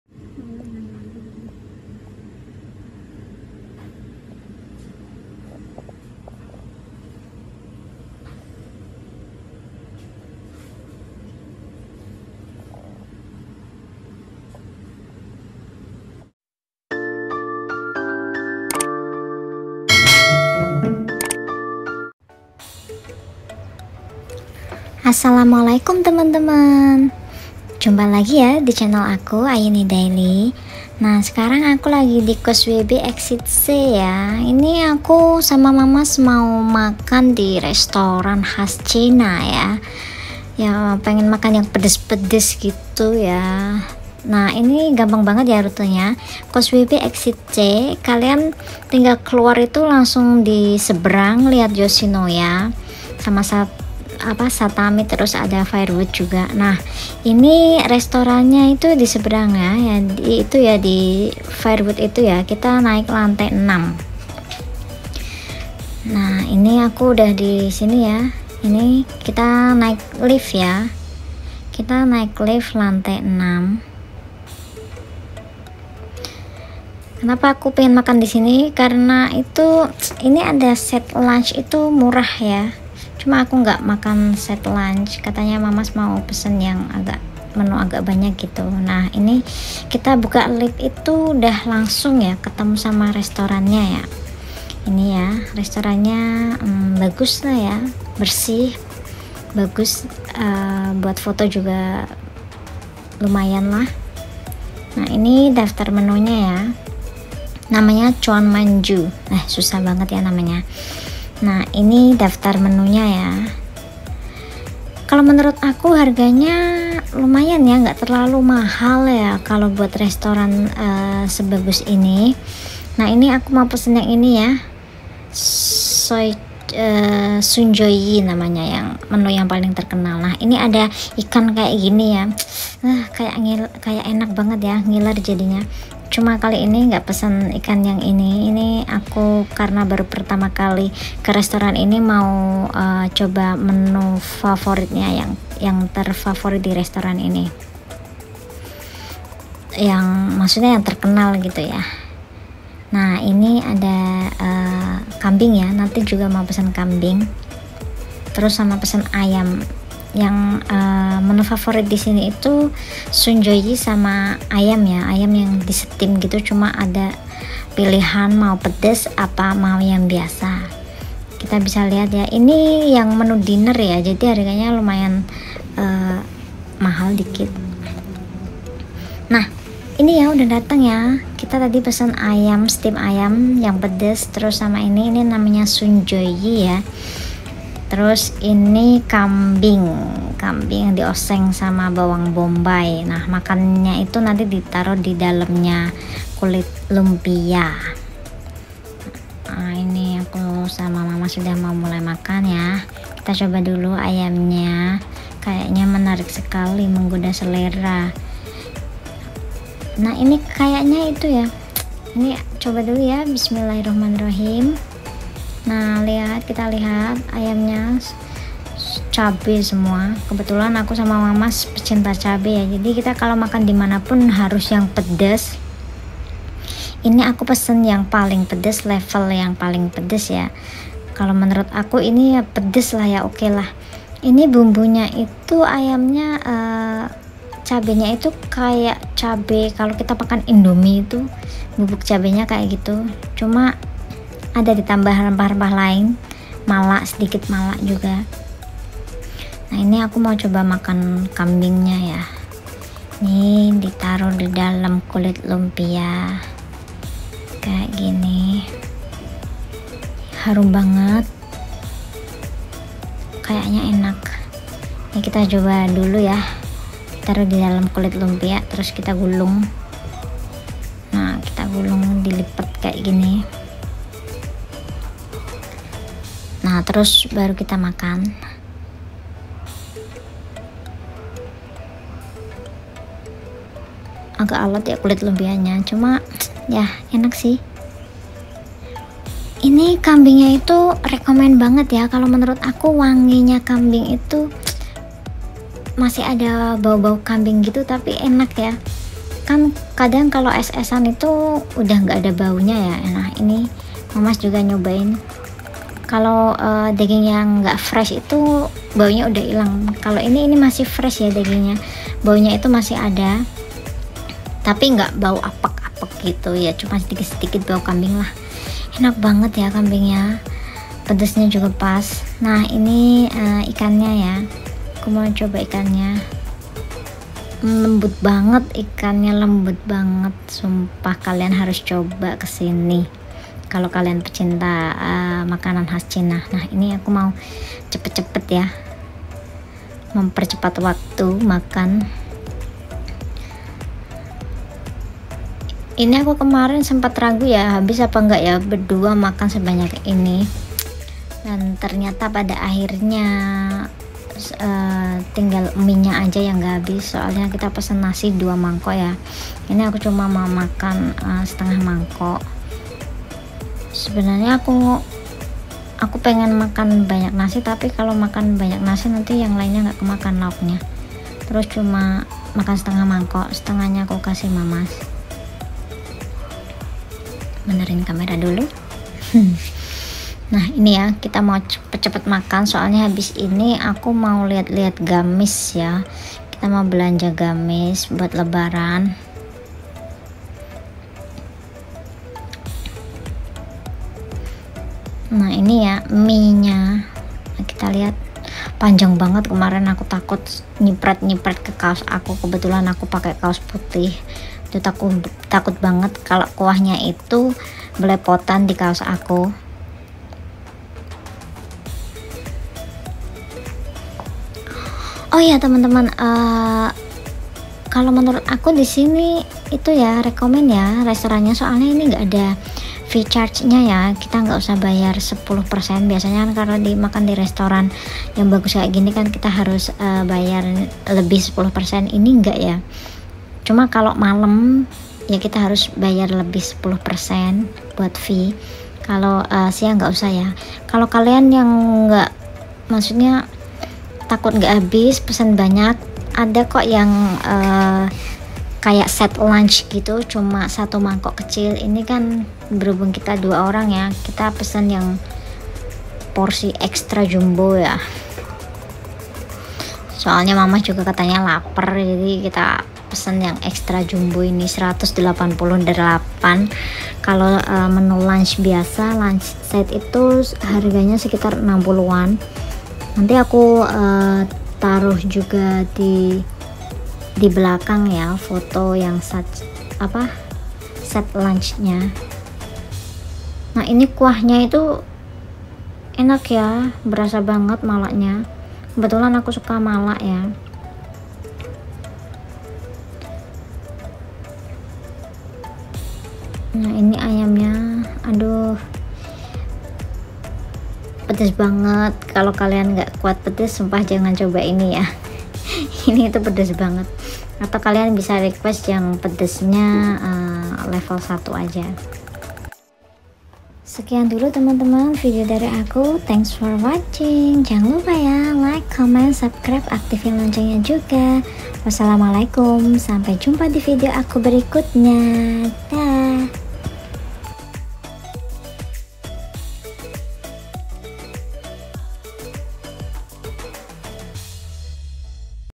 ChuanManju restoran khas China pecinta pedas wajib nyoba
Di video kali ini aku Di causewaybay
Mampir Di restoran khas China
ChuanManju yang berlokasi dekat banget dengan mtr exit c
Menunya endilita banget harganya juga sesuai kantong
Pedasnya bikin ketagihan
Teman teman pecinta pedas yang ada Di hongkong jangan lupa mampir ya
Nggak nyesel pokoknya
Langsung aja ya kita lihat videonya